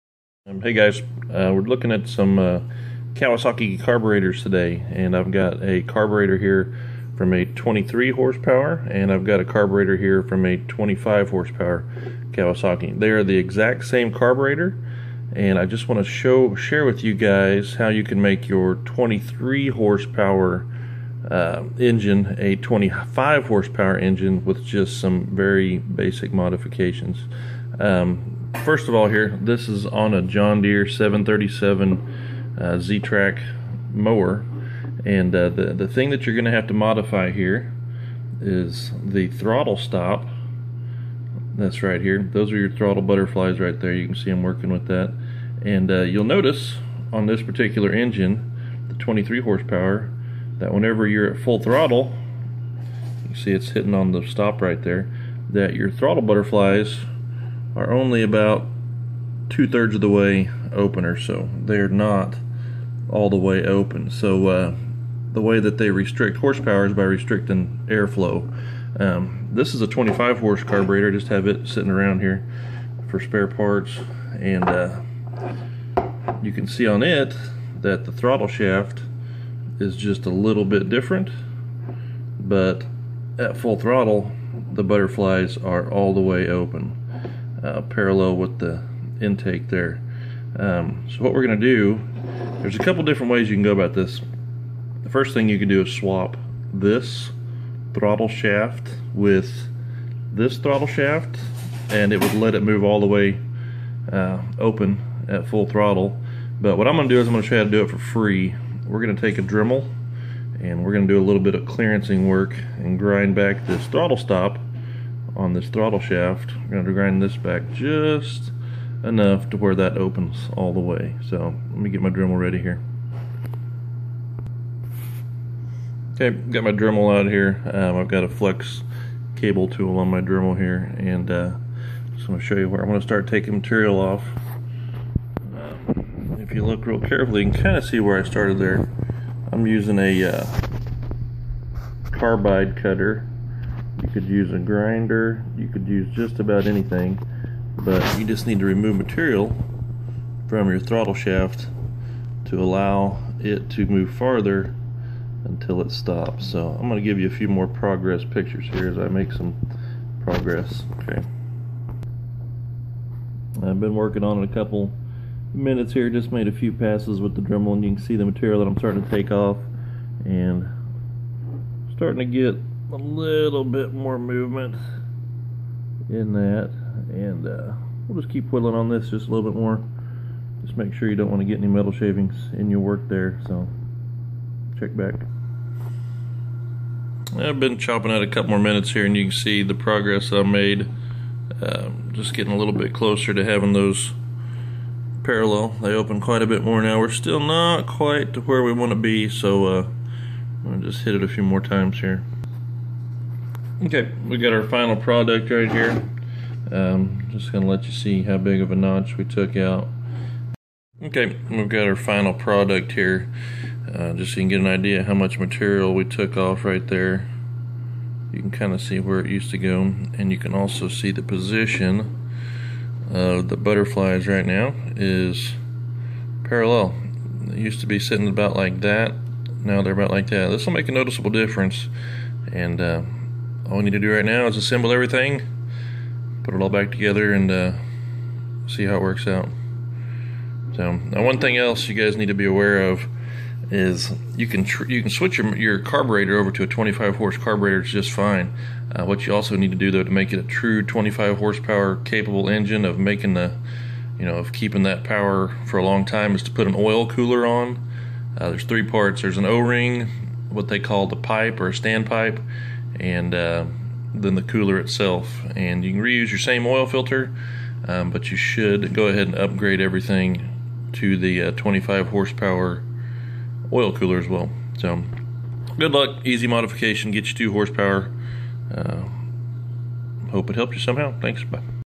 John Deere 737 757 717 Kawasaki FH601D, FH641D, FH680D upgrade to 25HP, FH721D. Part 1
Quick directions on how to increase the horsepower on your 19, 21, or 23HP FH601, FH641, or FH680D engine to 25HP. On the 25HP engine, there is also an oil cooler that needs to be added to help keep the oil cool.